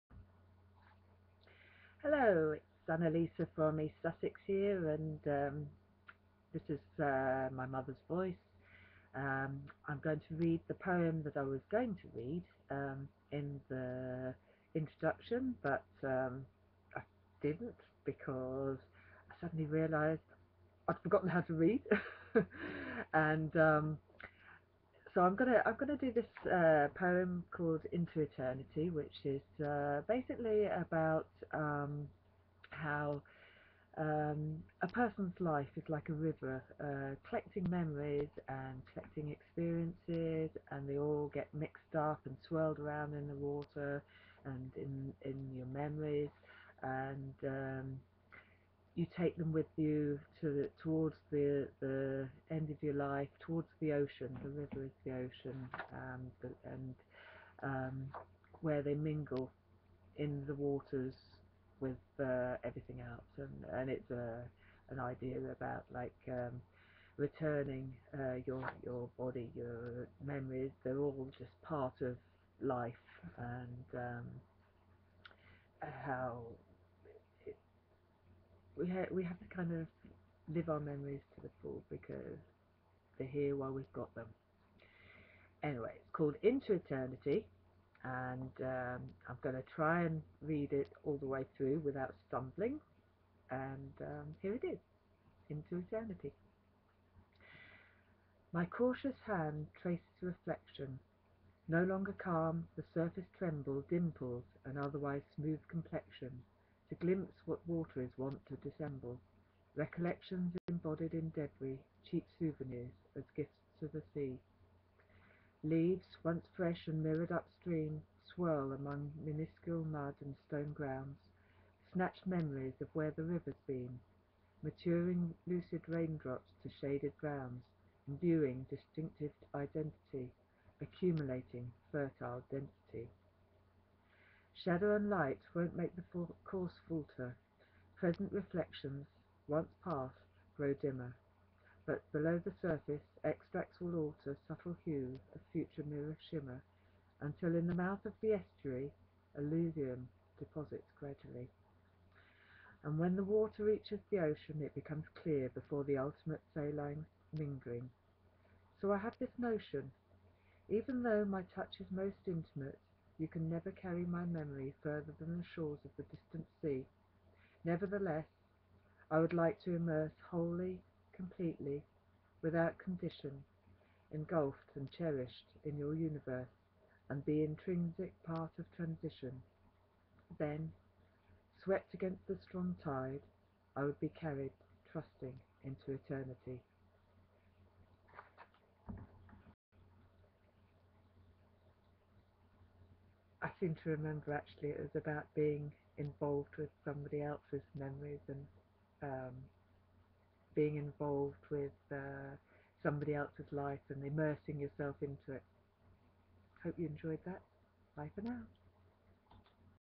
My Mother's Voice: "Into Eternity"
Into Eternity
by Annelisa Christensen

Thinking about how we flow through life,  picking up memories and experiences, on the way to final release when we finally become one with the sea of eternity once more.